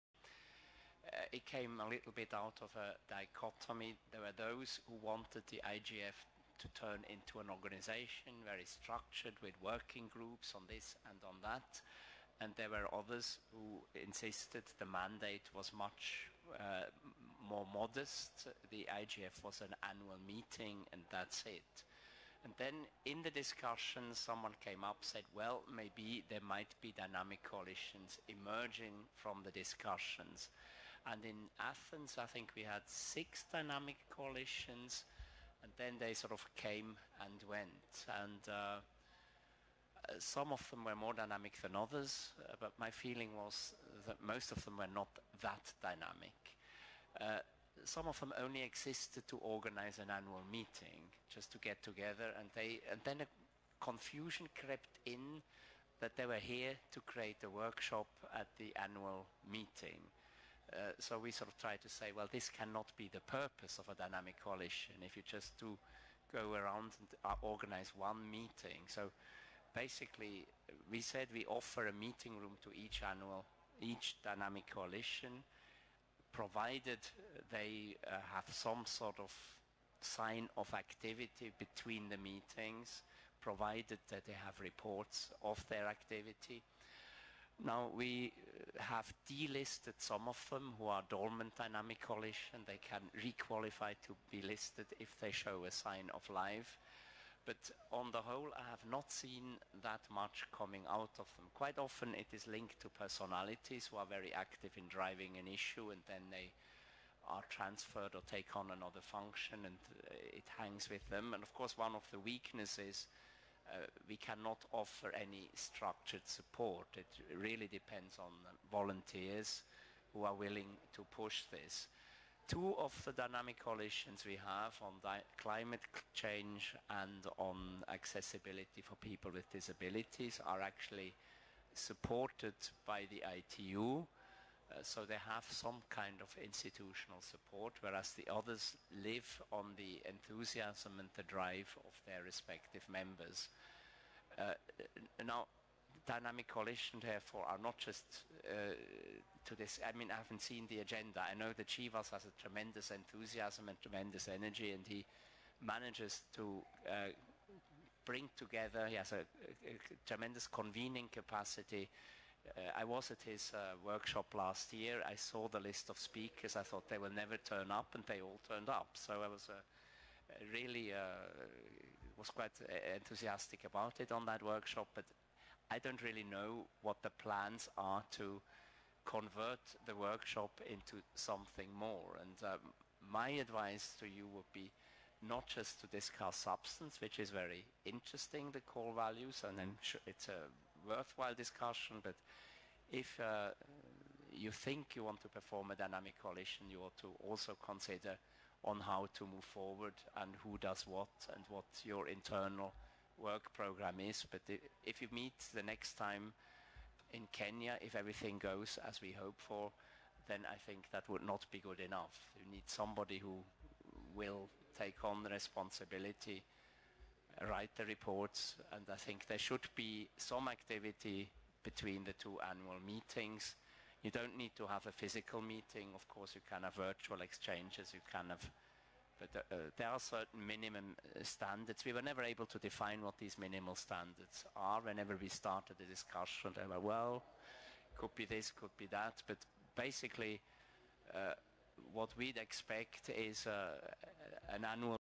Global IGF 2010: Markus Kummer
Markus Kummer, leader of the UN's secretariat for the Internet Governance Forum, continues the explanation of the development of Dynamic Coalitions and tells about the requirements IGF organizers have set for DCs in order to have meeting space reserved for them at the global IGF.